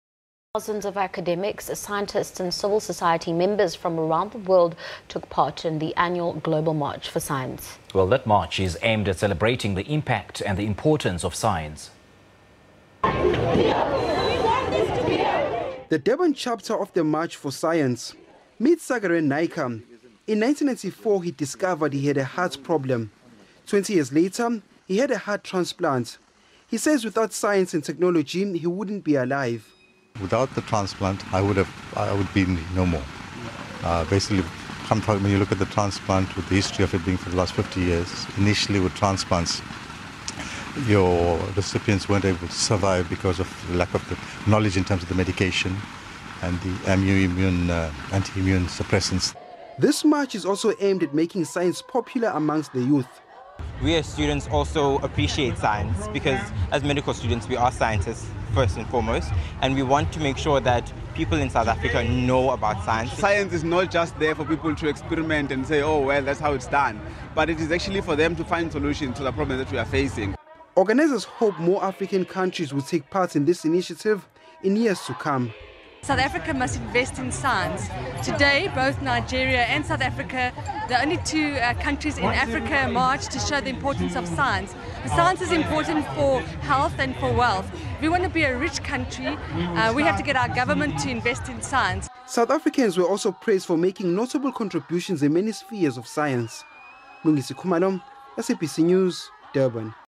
Thousands attend the annual Global March for Science
Thousands of academics, scientists and civil society members from around the world took part in the annual Global March for Science.
The march is aimed at celebrating the impact and importance of science.